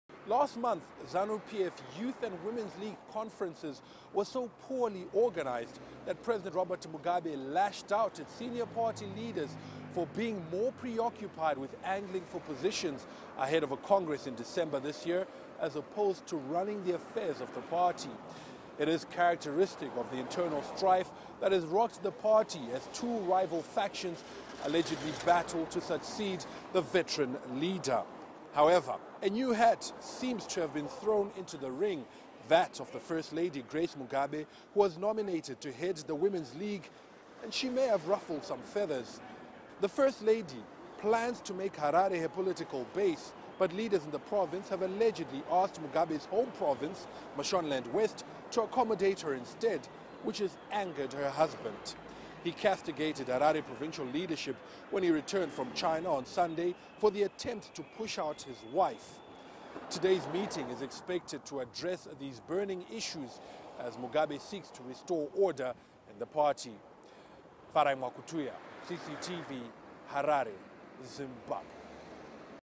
President Mugabe Calls For ZANU-PF Emergency Meeting
Zimbabwe's President Robert Mugabe has called an emergency meeting of senior officials in his ruling party.That comes amid reports of rifts within ZANU-PF.CCTV's Farai Mwakutuya reports